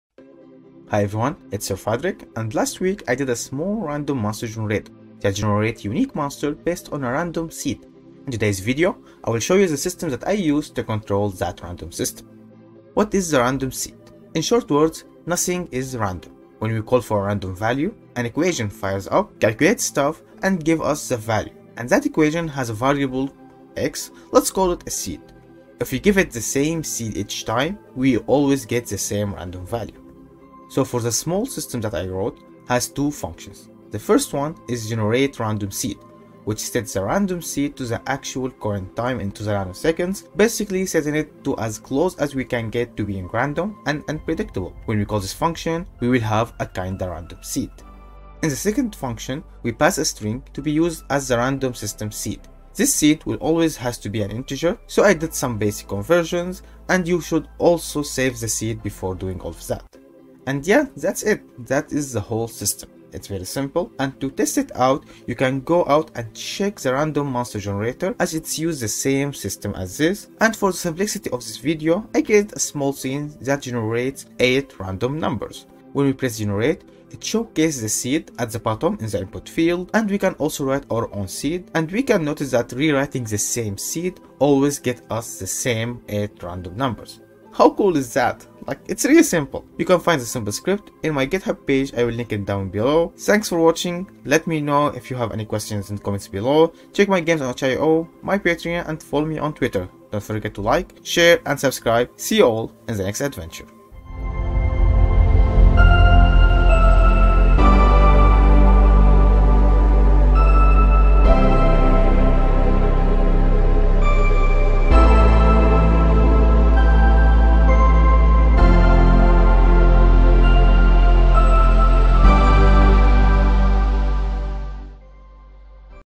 A Simple Random Seed System | Unity Tutorial
Hi everyone,
A simple random seed System that can be used in any game that requires seed randomization and recovering.